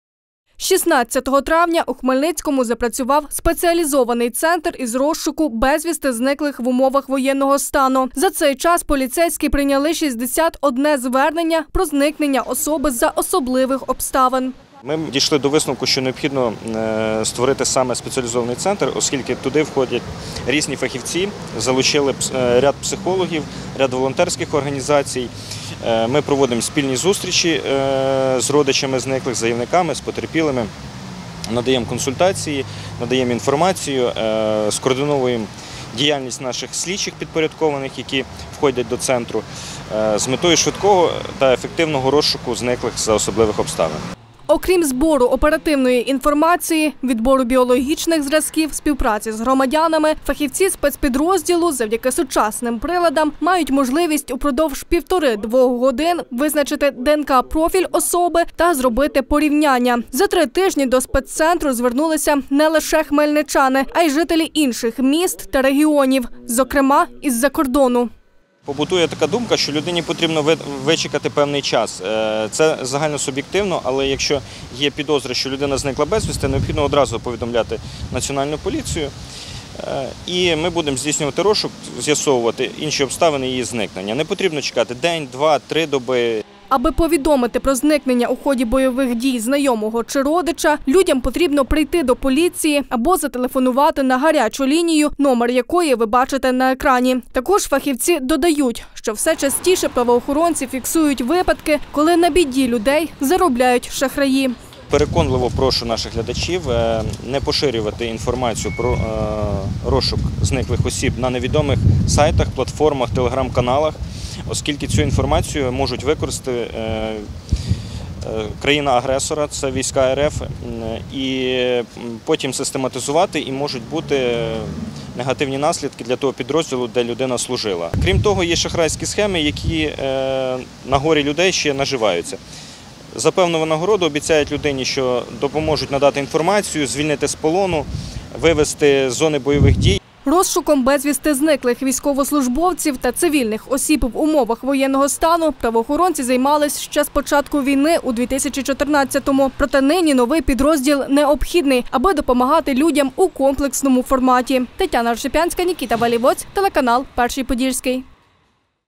Безвісти зниклі – скільки таких у Хмельницькому?
У поліції Хмельниччини із 16 травня запрацював спеціалізований центр із розшуку безвісти зниклих в умовах воєнного стану. За цей час поліцейські прийняли 61 звернення про зникнення осіб, та відібрали 77 ДНК-зразків. Детальніше про роботу фахівців – дивіться у сюжеті.